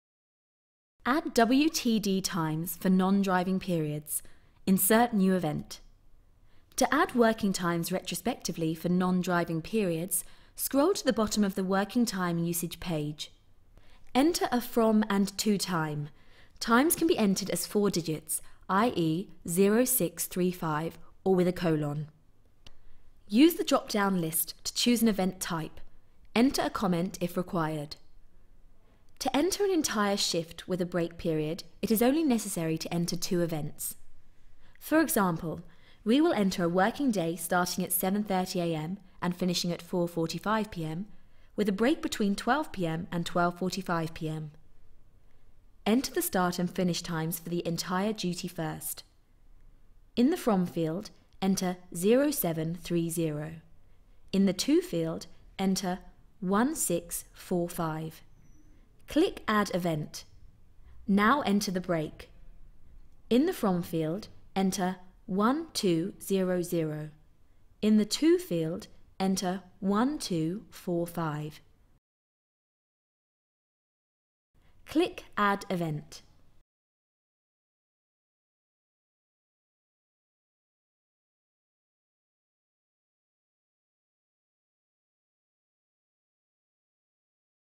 Add Non Driving Duty - Insert New Event - Tachomaster Tachograph Analysis
- Tachomaster - the market leading Tachograph Analysis solution. This tutorial explains how to add a single working time event into the Tachomaster system.